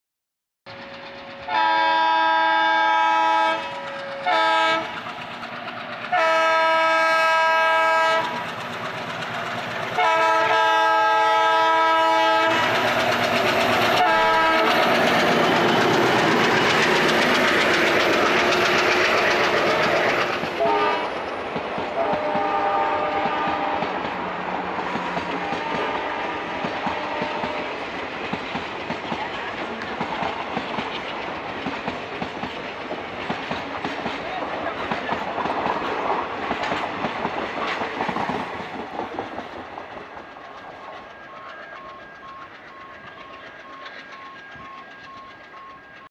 Massive Offlink!! 13465 Up Howrah-Malda Town Intercity Express With NGC WDM 3A #16488
13465 Up Howrah - Malda Town Intercity Express (via: Katwa-Azimganj)
হাওড়া - মালদা টাউন ইন্টারসিটি এক্সপ্রেস (ভায়া:  কাটোয়া-আজিমগঞ্জ)
हावड़ा - मालदा टाउन इंटरसिटी एक्सप्रेस (वाया: कटवा-अज़ीमगंज)

➡Total Time: 7hrs 5mins
➡Halts: 13
➡Distance: 332 K.M
➡Speed: 57 K.M./Hour
➡Train Type: Mail/Express
➡Zone: ER/Eastern

This train departs from Howrah at 3:15pm and reaches to its destination at 10:20pm.